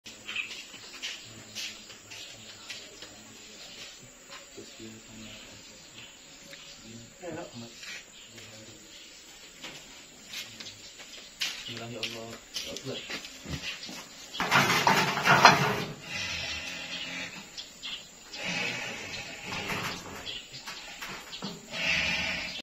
Proses penyembelihan Kambing aqiqah baby Zaskia
catering by montala aqiqah